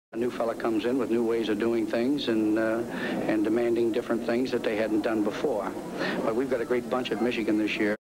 1969 Bo Schembechler on New Team
New Michigan Football Coach Bo Schembechler talks about his new team during this 1969 interview.